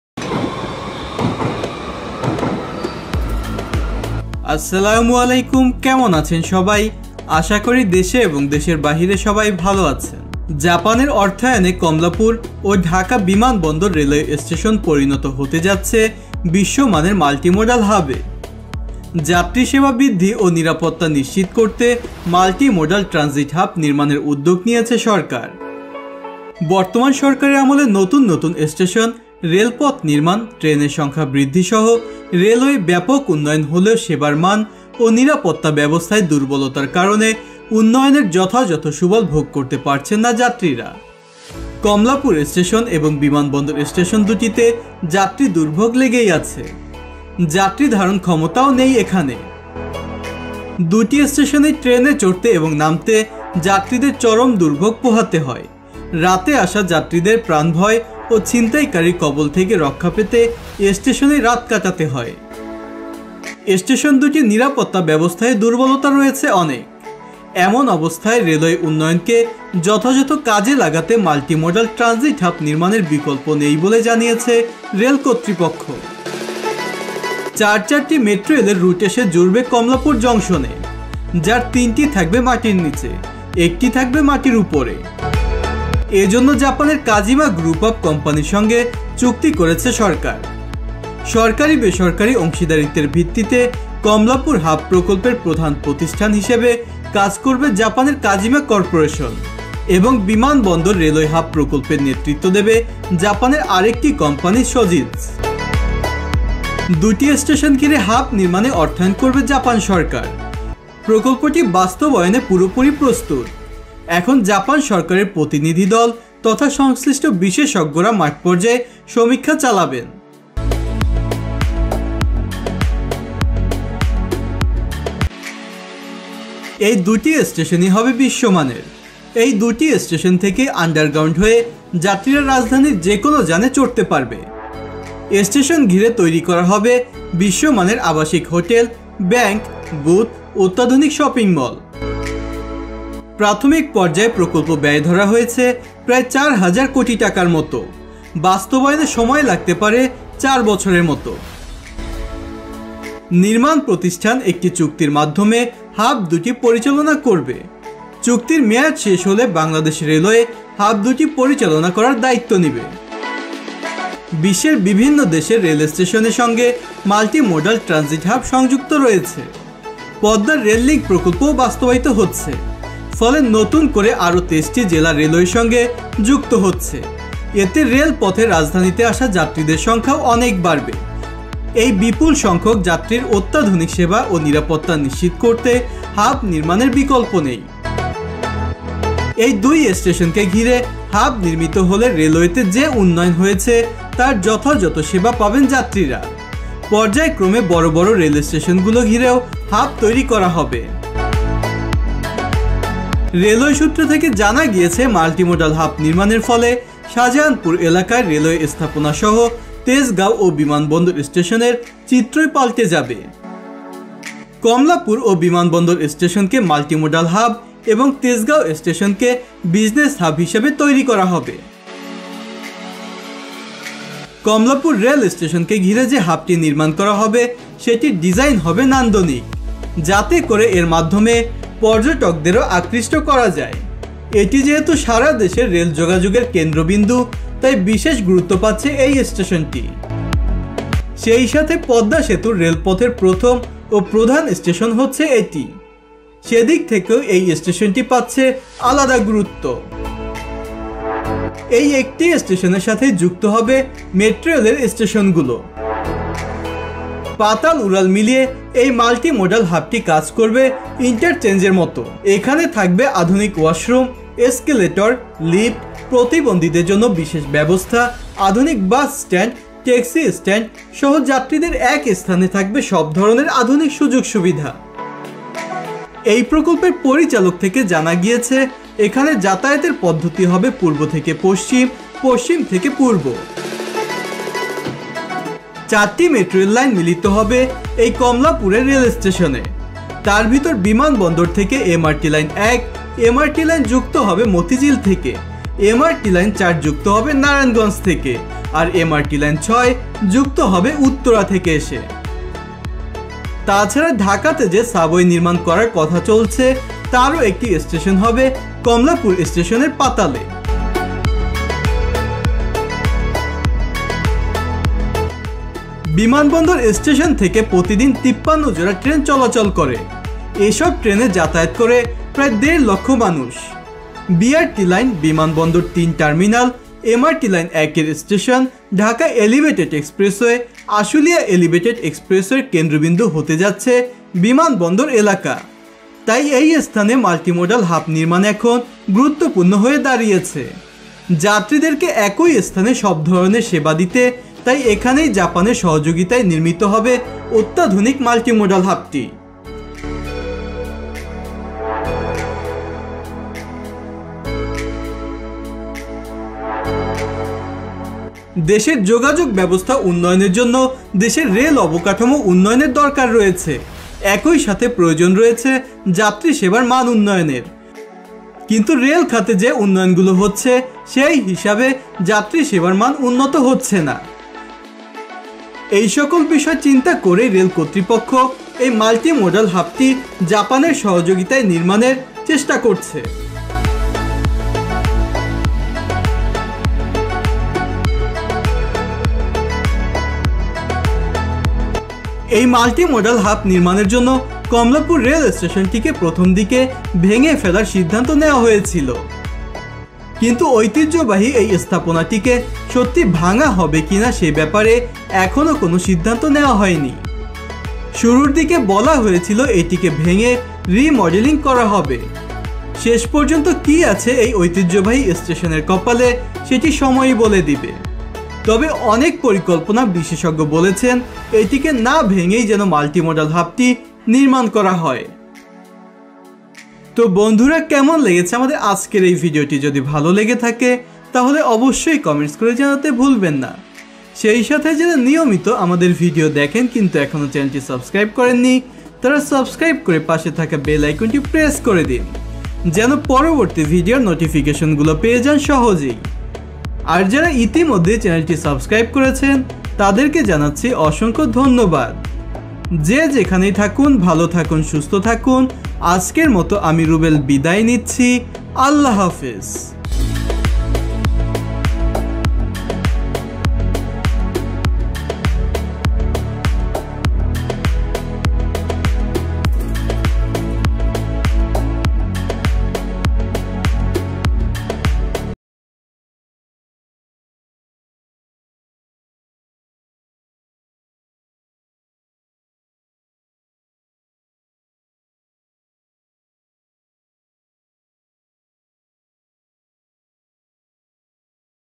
জাপানের হাতে ঢাকায় নির্মাণ হচ্ছে বাংলাদেশের প্রথম ২টি মাল্টিমোডাল হাব | Multimodal Hubs at Dhaka
প্রথম বারের মতো ঢাকায় নির্মাণ হচ্ছে মাল্টিমোডাল হাব । Multimodal Hub at Dhaka । Uplift Bangladesh

জাপানের হাতে ঢাকায় নির্মাণ হচ্ছে বাংলাদেশের প্রথম ২টি মাল্টিমোডাল হাব | Multimodal Hub at Dhaka | Uplift Bangladesh

জাপানের অর্থায়নে বিশ্বের অন্যতম মাল্টিমোডাল হাব হচ্ছে ঢাকার। কমলাপুর ও  বিমানবন্দর রেলওয়ে স্টেশনে । কমলাপুরে ১৫৬ একর জমিজুড়ে তৈরি হওয়া এই মাল্টিমোডাল হাব-স্টেশন ভবনে থাকবে ৫ তারকা হোটেল, যাত্রীদের জন্য সুসজ্জিত অপেক্ষাগার, সব ধরনের শপিংমল, হাসপাতাল এমনকি বিনোদনের ব্যবস্থাও। চার চারটি মেট্রোরেলের রুট এসে জুড়বে কমলাপুর জংশনে।

যার ৩টি থাকবে মাটির নিচে, একটি থাকবে মাটির উপরে। এজন্য জাপানের কাজিমা গ্রুপ অব কোম্পানিজের সঙ্গে চুক্তি হয়েছে সরকারের। সরকারি-বেসরকারি অংশীদারিত্বের (প্রাইভেট পাবলিক পার্টনারশিপ-পিপিপি) ভিত্তিতে কমলাপুর হাব প্রকল্পের প্রধান প্রতিষ্ঠান হিসেবে কাজ করবে কাজিমা এবং বিমানবন্দর রেলওয়ে হাব প্রকল্পের নেতৃত্ব দেবে সজিৎজ।

এয়ারপোর্ট মাল্টিমোডাল হাব কনসেপ্ট ডিজাইন ক্রেডিট : Ahmed Real